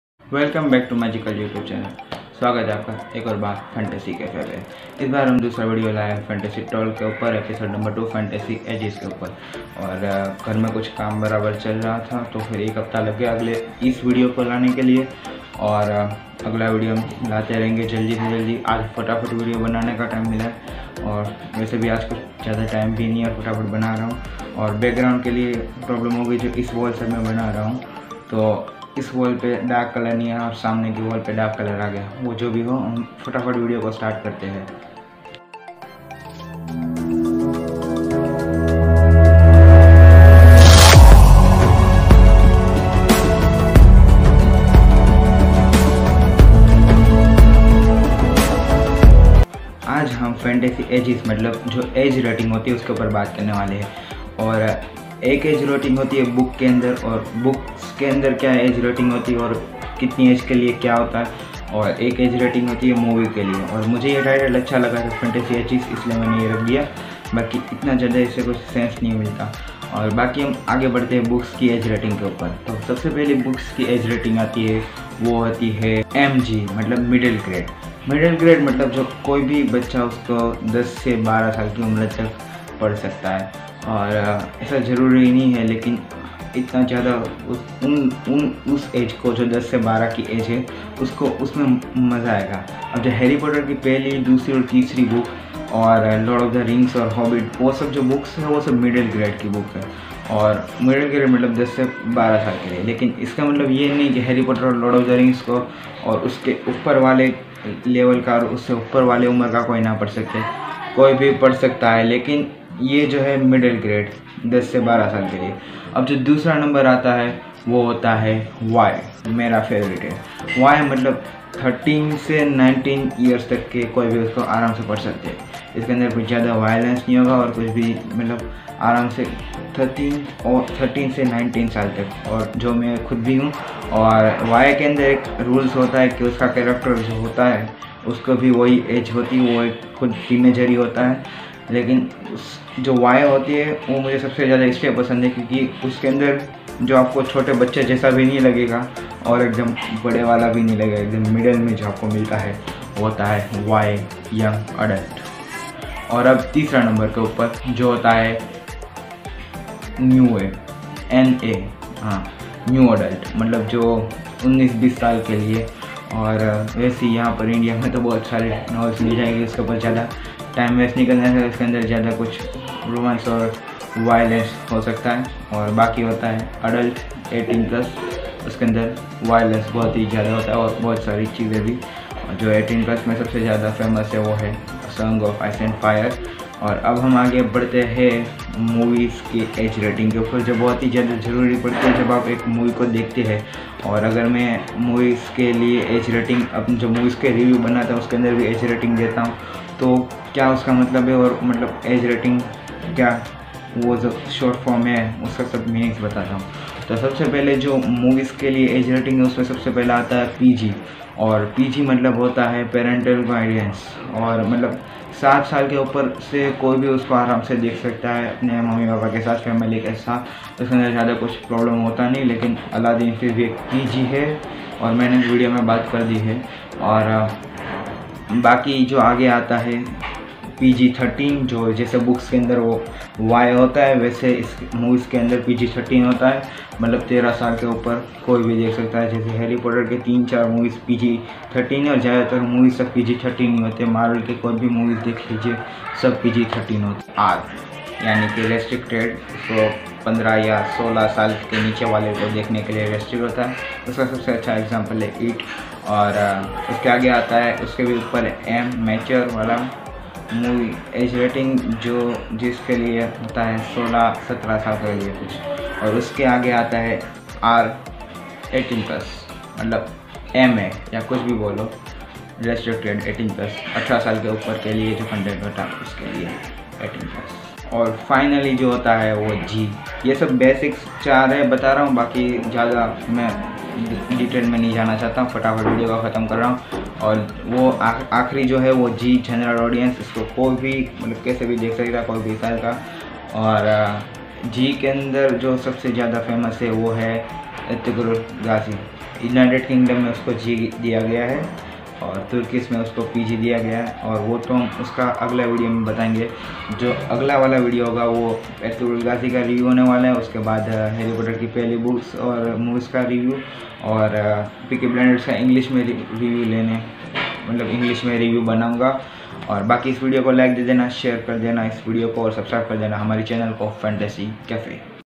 Fantasy Ages - Fantasy Talk ep 2
Here I am explained about Age Rating In Books and Movies.